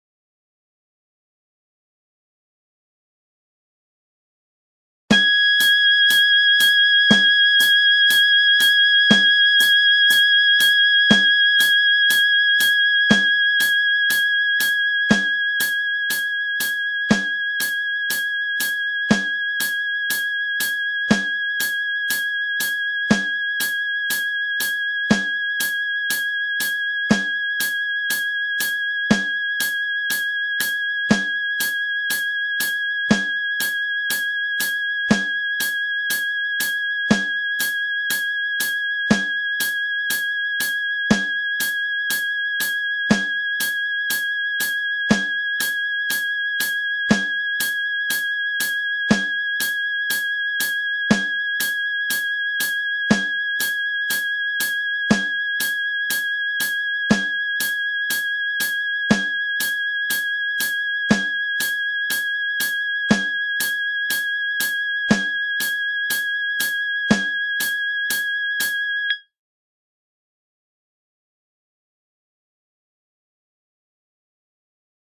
044 F-4 1661.22 Hz by Rex Djere
This is a simulated alto saxophone (generated using MuseScore) playing the alto saxophone's F-4 note at a frequency of 1661.22Hertz.

Please visit the Saxtalk.com Alto Saxophone Fingering Charts for more information on the four octaves of notes playable on the alto saxophone: